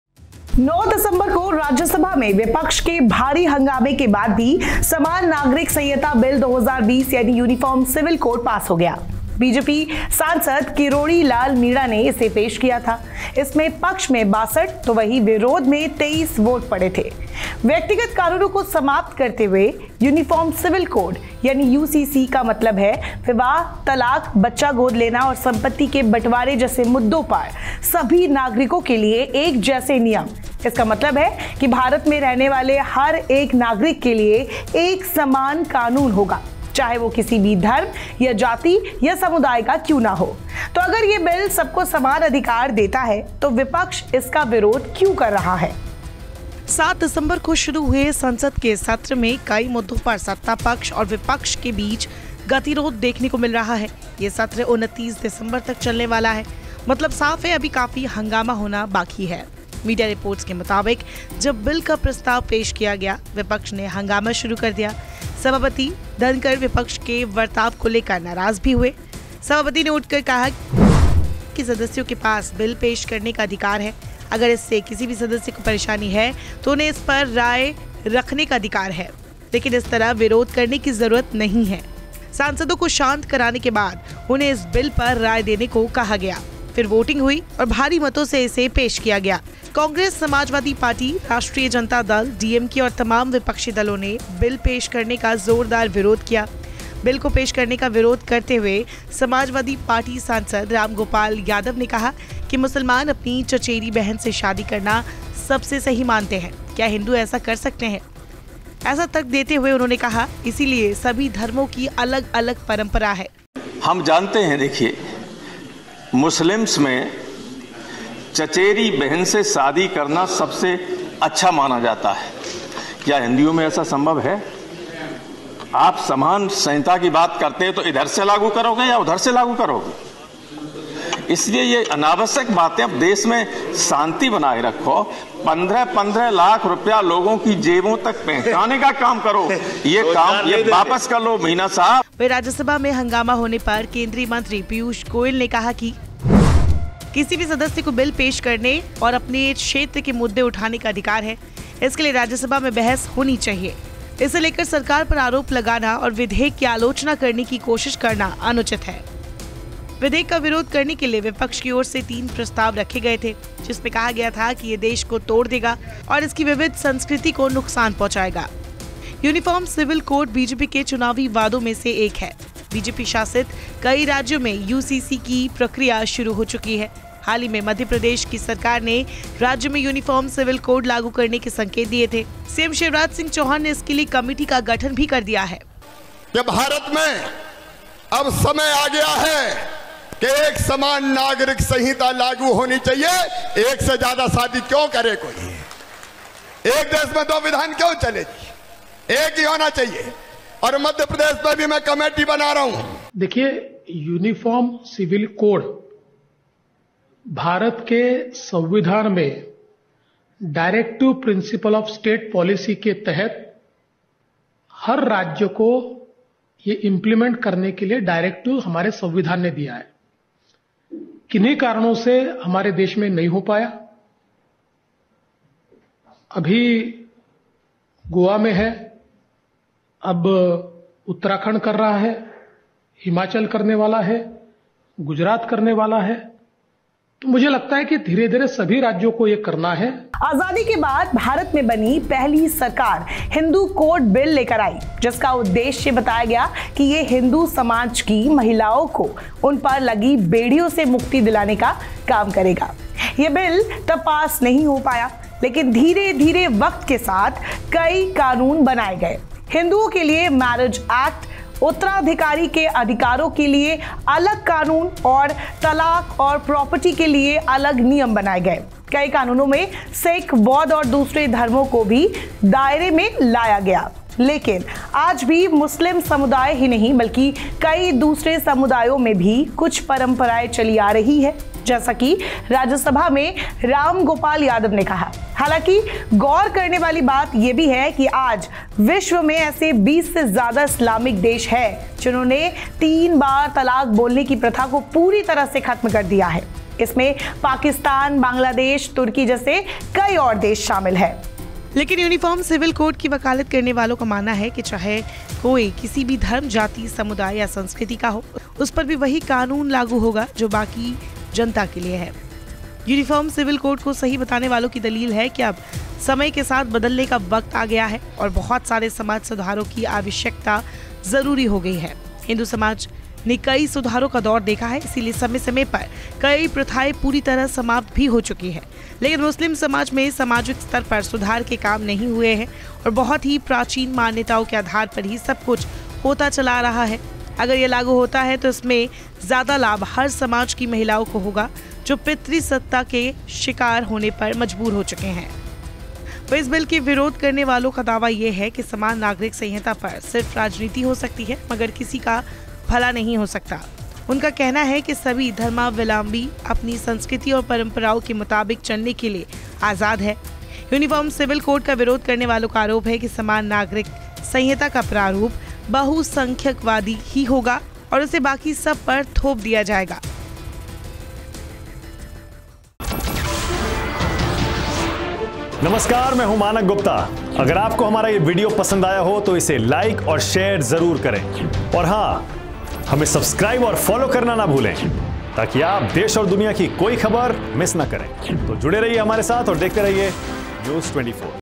हंगामे के बीज Rajyasabha में BJP सांसद ने पेश किया बिल,क्या है Uniform Civil Code? क्या है विवाद?
9 दिसंबर को राज्यभा में विपक्ष के भारी हंगामे के बाद भी समान नागरिक संहिता बिल 2020 यानी यूनिफॉर्म सिविल कोड पास हो गया...भाजपा सांसद किरोड़ी लाल मीणा ने इसे पेश किया था...इसमें पक्ष में 62 वहीं विरोध में 23 वोट मिले...व्यक्तिगत कानूनों को समाप्त करते हुए, यूनिफॉर्म सिविल कोड (UCC) का मतलब है विवाह, तलाक, बच्चा गोद लेना और संपत्ति के बंटवारे जैसे मुद्दों पर सभी नागरिकों के लिए एक जैसे नियम. इसका मतलब है कि भारत में रहने वाले हर एक नागरिक के लिए एक समान कानून होगा, चाहे वह किसी भी धर्म या जाति का क्यों न हो. तो अगर ये बिल सबको समान अधिकार देता है...तो विपक्ष इसका विरोध क्यों कर रहा है?